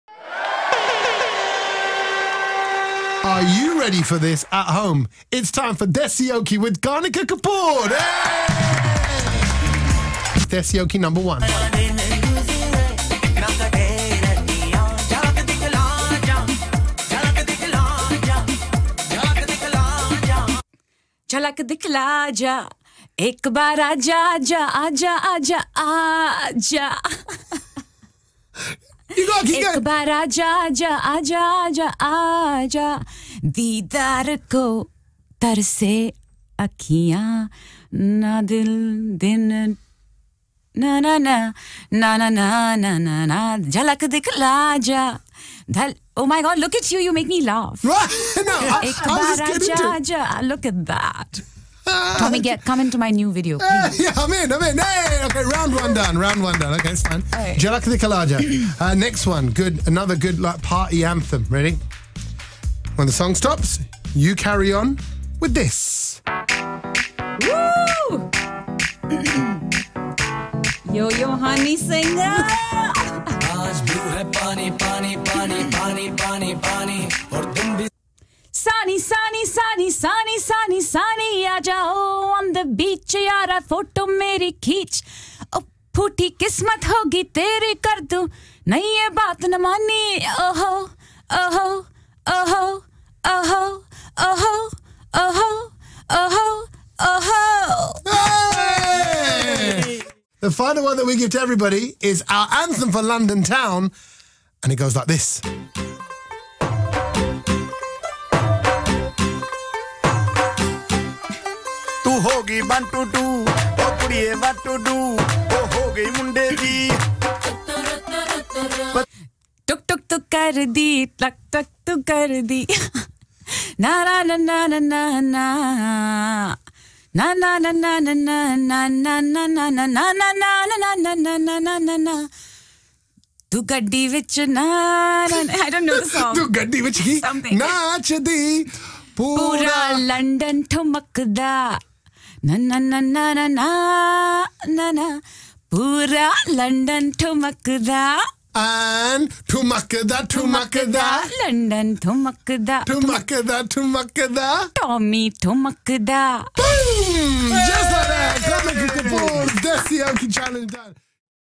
Tommy Sandhu: Desioke with Kanika Kapoor
The lovely Kanika Kapoor accepted the Desioke Challenge.

Don't forget the Baby Doll will be performing at BBC Asian Network Live on Saturday 30th April at the Eventim Apollo, Hammersmith.

You can be there!